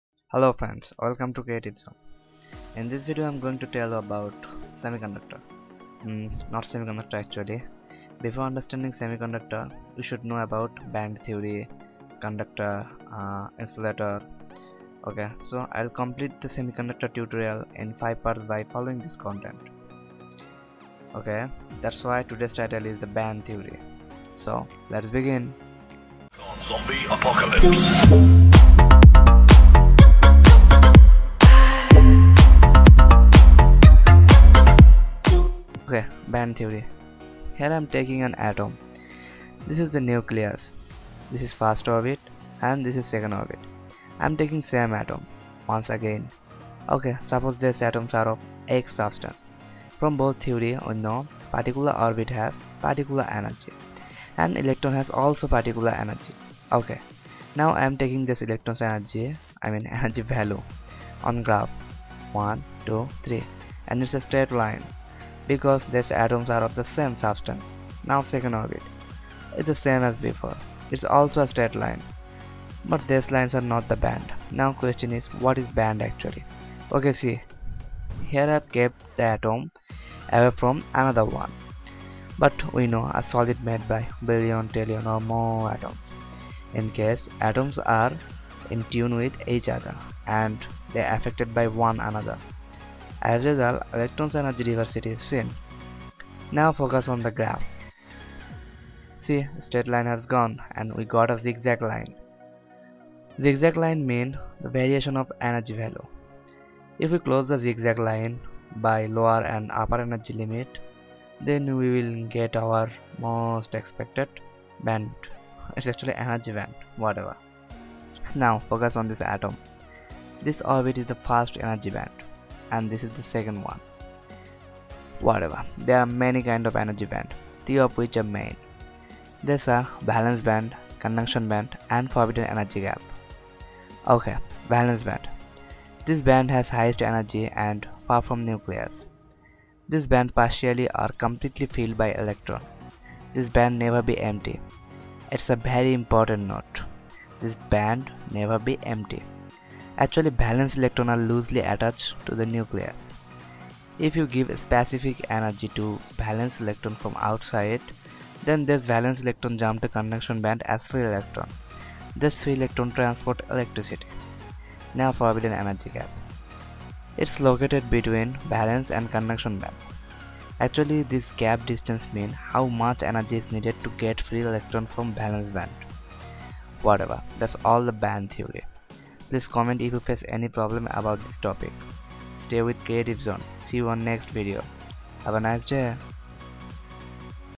Band Theory | Semiconductor Explained | Part-01
Band Theory- You must have understand this theory otherwise you can not understand semiconductor properly.
1st you have to notice structure of an atom.We know particular orbit has particular energy and electron also has energy to spin their own orbit.If You take some atom of same substance then you get same energy value of electrons (Electrons are must be in same orbit).But We know any kind of solid made by countless atoms and these atoms are connected with each other and affected by one another.As result electrons of these atoms create an energy variation.If we close these variation by lower and upper energy limit then we will get energy band.There are many kind of energy band but three of which are main.These are valence band , conduction band and forbidden energy gap.

Valence band,This band has heights energy and its the outter orbit of an atom.So that valence orbit far from neucleas.This band partially or completely filled.This band/Orbit never be empty.If you give particular energy to valence electrons then the valence electrons jump to conduction band as free electron.Now forbidden energy gap.this free space/gap mean how much enery is needed to get free electron from valence electron.

Thats All.